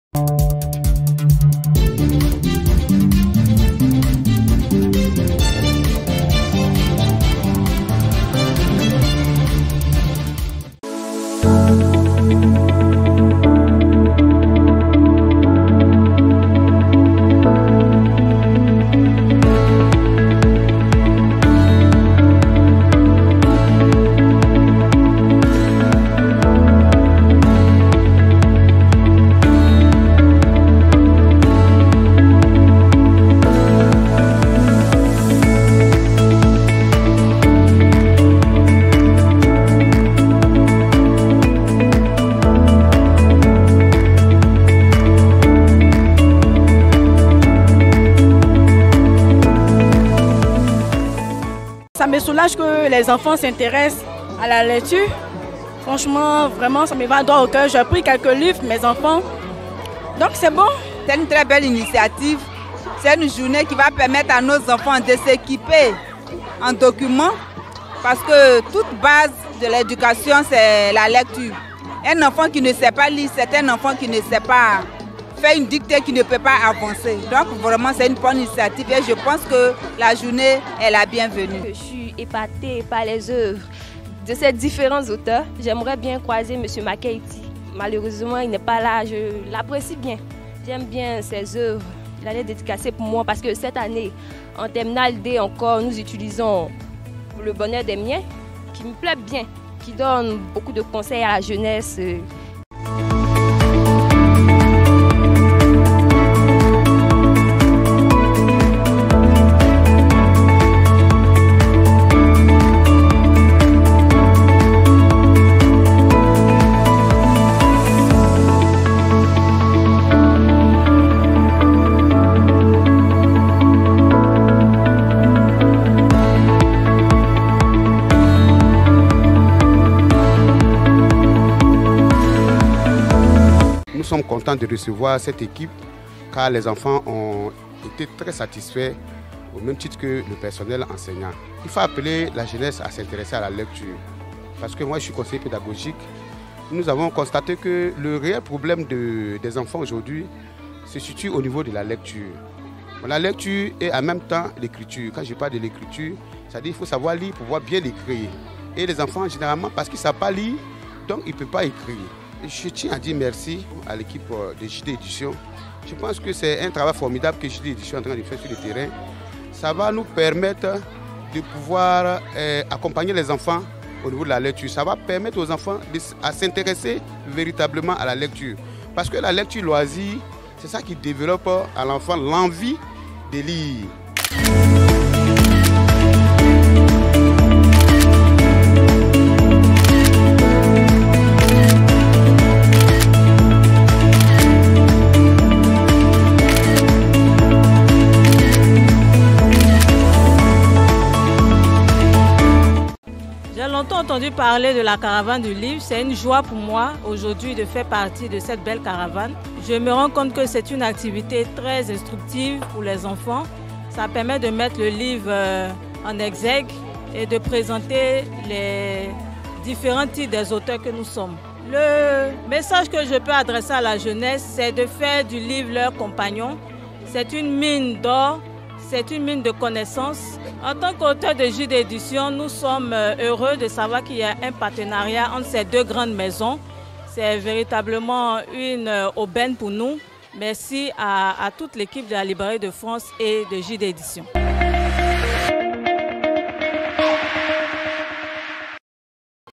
🔴Caravane du livre 2023 /Dabou : Les auteurs de JD Éditions communient avec la Librairie de France
Après la cérémonie du lancement officiel de la caravane du livre 2023 en Côte d’Ivoire qui s’est déroulée le mardi 31 octobre 2023 à l’institut français au plateau, s’est tenue dans la journée du mercredi 08 Novembre 2023 à Dabou au Groupe Scolaire Nayeba et au Groupe Scolaire Catholique Mixte la première journée de ladite caravane placée sous le thème : « La valorisation des femmes à travers les livres », avec le sous thème, « lire libère. »